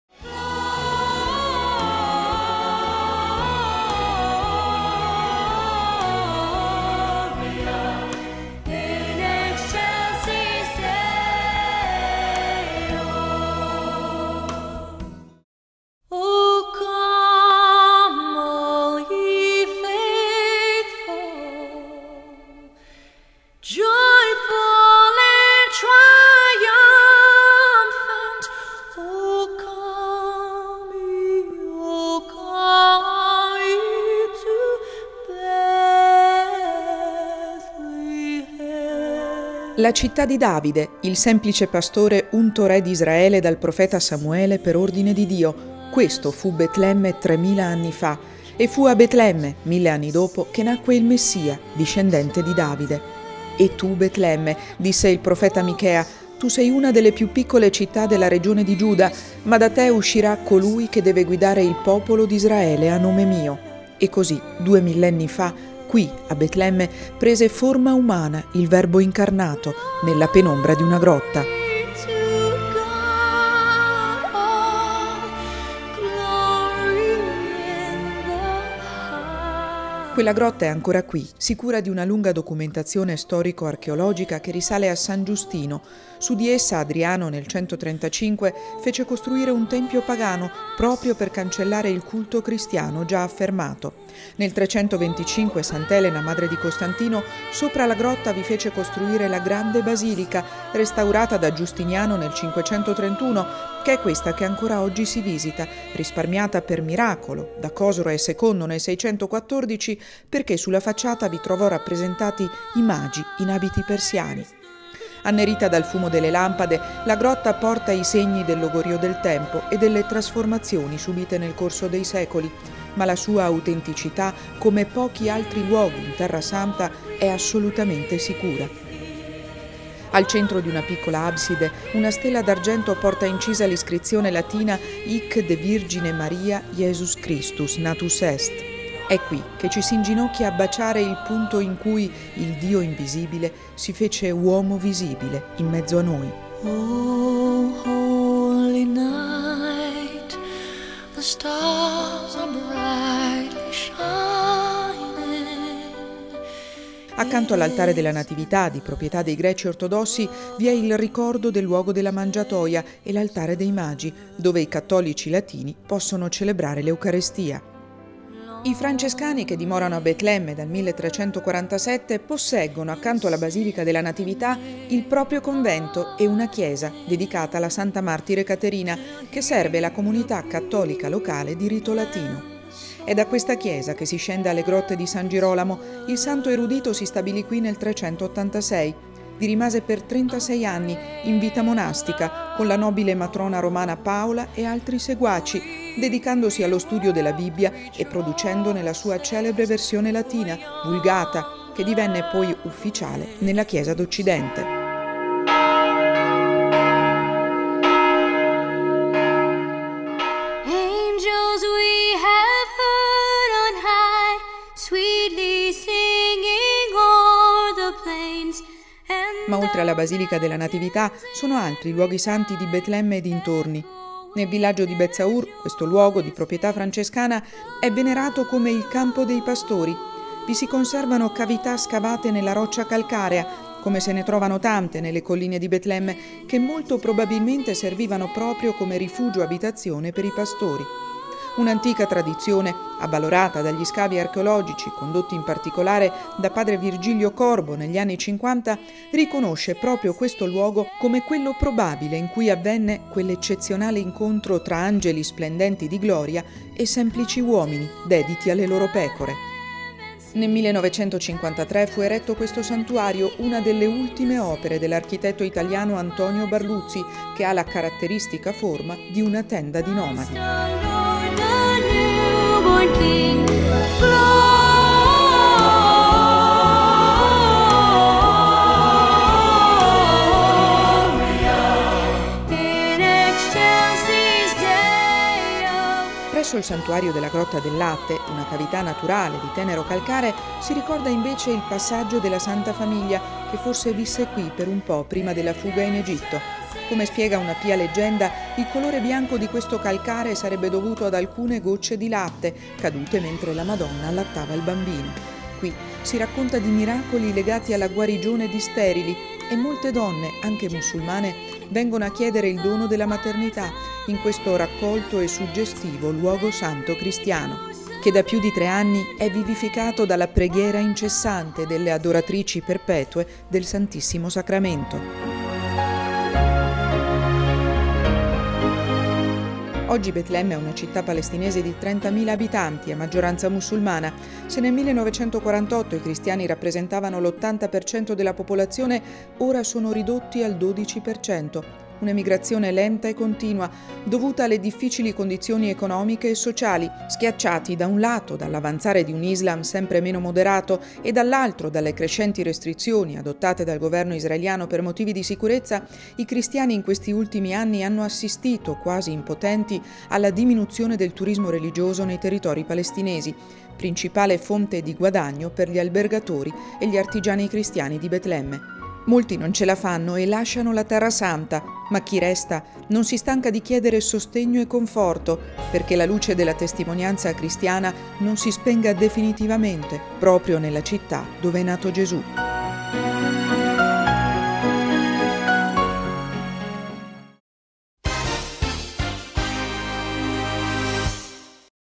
E tu Betlemme
luoghi santi e pietre vive ... un viaggio, tra passato e presente, nella piccola citta' di Giuda